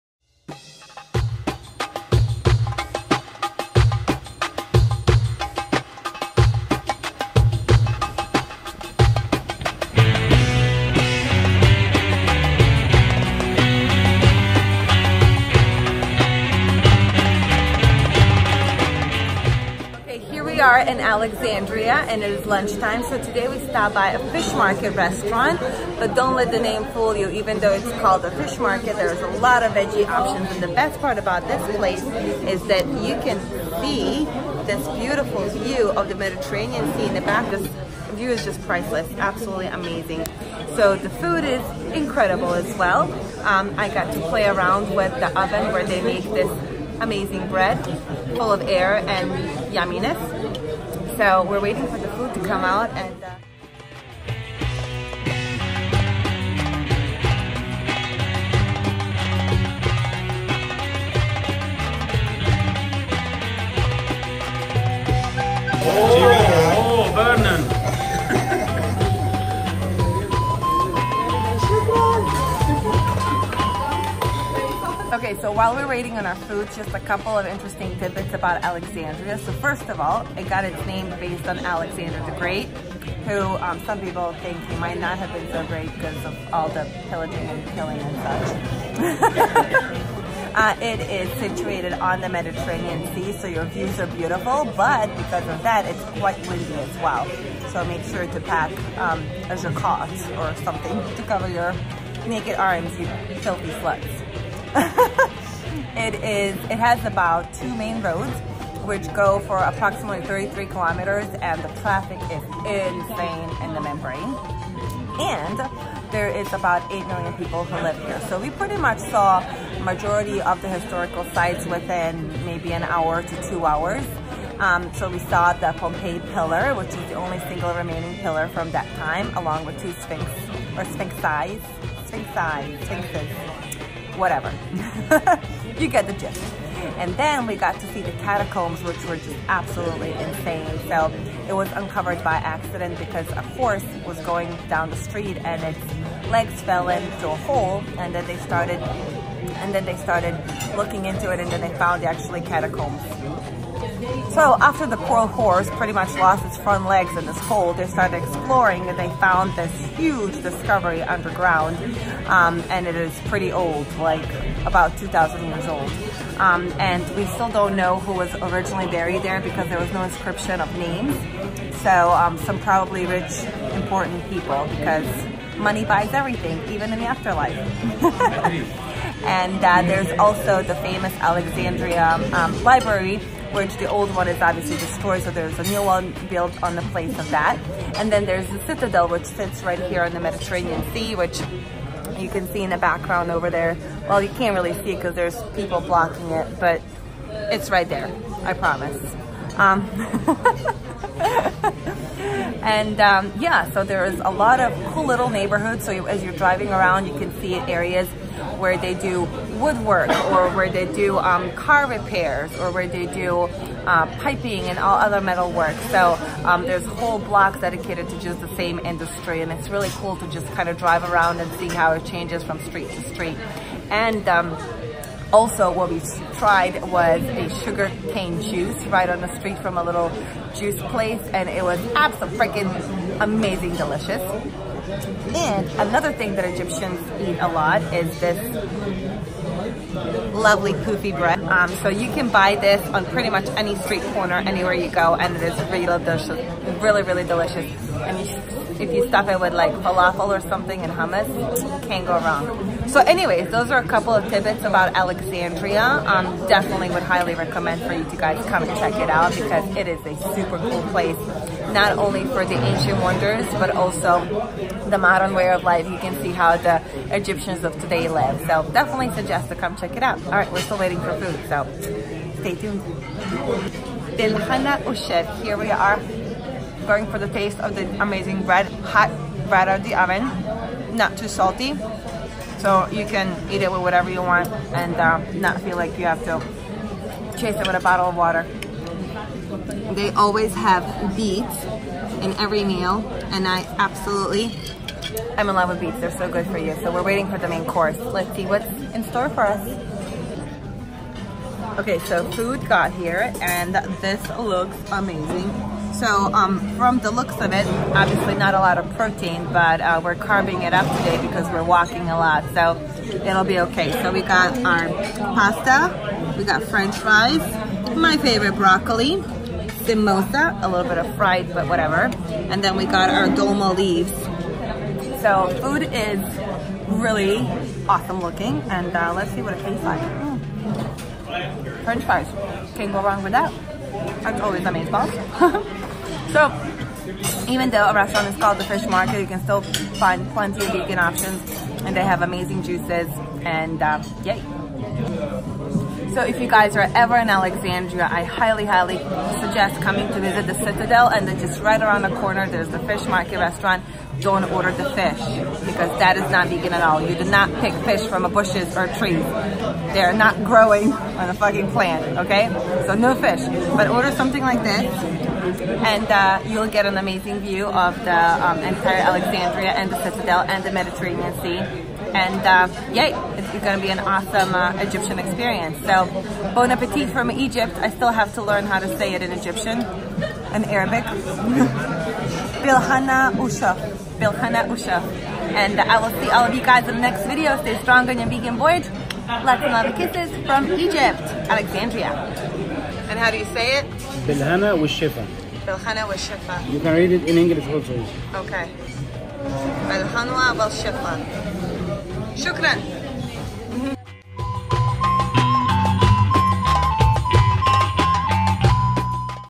Crazy Vegan Adventures In Alexandria, Egypt (Ooonnnn Fiiiiiireeee - Literally)
Alexandria is such a magical place. I have been studying the history of this region since my childhood... and it has been such a surreal experience to finally go see it in real life. To walk in the footsteps of Alexander The Great. an absolute surreal experience.
Visiting the historic sites was like an absolute dream come true.
My vegan food adventures in Alexandria were an absolute blast!!!